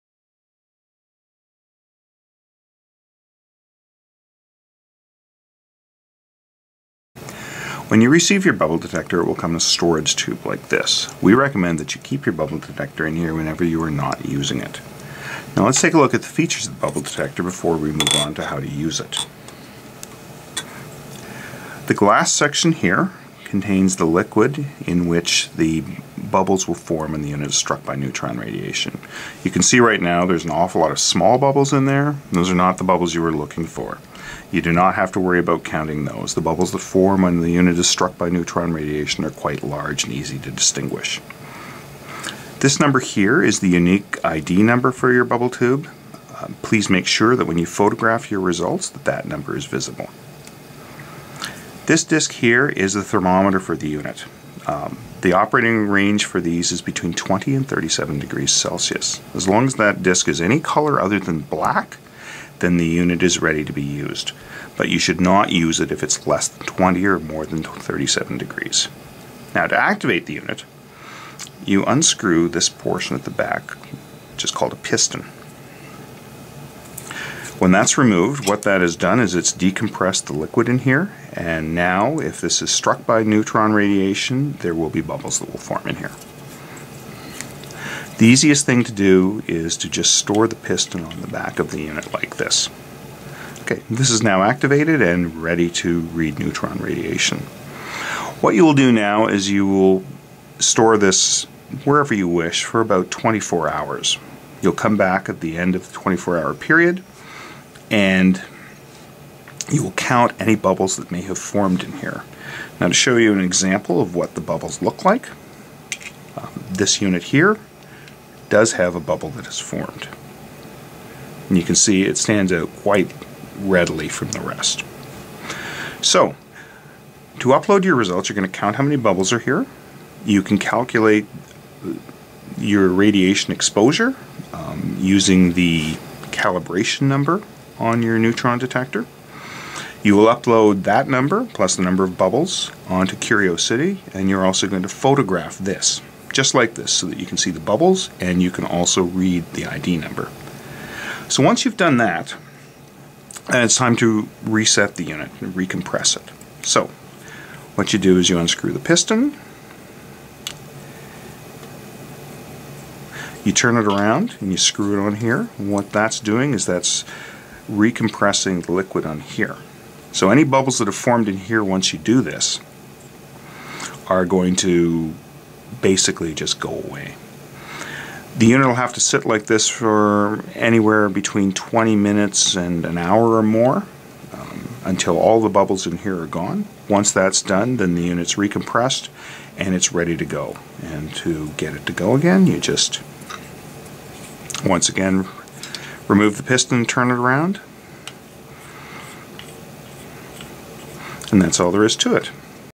How to use a bubble detector - personal neutron dosimeter
This video shows you how to use a bubble detector as part of the RADI-N2 Action Project.